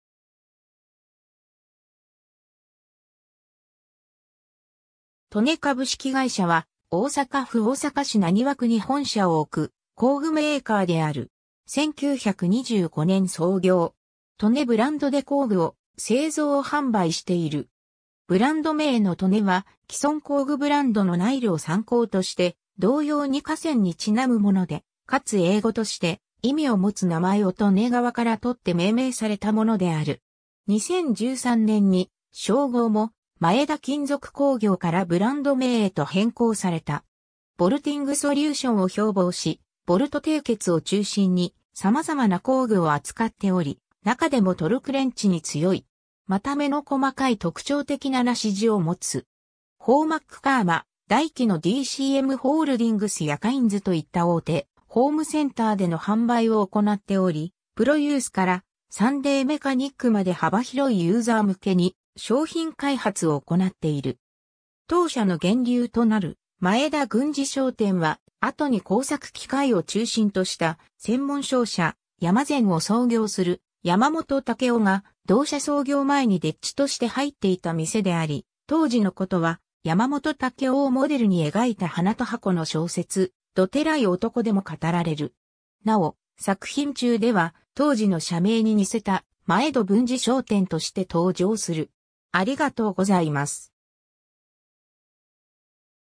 TONE (企業)
TONE株式会社（とね、英: TONE CO., LTD.）は、大阪府大阪市浪速区に本社を置く工具メーカーである。
1925年（大正14年）創業。
 「TONE（トネ）」ブランドで工具を製造販売している。
ブランド名のTONEは既存工具ブランドのNileを参考として同様に河川に因むもので、かつ英語として意味を持つ名前を利根川からとって命名されたものである。
2013年に商号も「前田金属工業」からブランド名へと変更された。
 「ボルティング・ソリューション」を標榜し、ボルト締結を中心に様々な工具を扱っており、中でもトルクレンチに強い。
また目の細かい特徴的な梨地をもつ。
 ホーマック・カーマ・ダイキのDCMホールディングスやカインズといった大手ホームセンターでの販売を行なっており、プロユースからサンデーメカニックまで幅広いユーザー向けに商品開発を行なっている。
 当社の源流となる「前田軍治商店」は、後に工作機械を中心とした専門商社・山善を創業する山本猛夫が同社創業前に丁稚として入っていた店であり、当時のことは山本猛夫をモデルに描いた花登筺の小説『どてらい男』でも語られる。
なお、作品中では当時の社名に似せた「前戸文治商店」として登場する。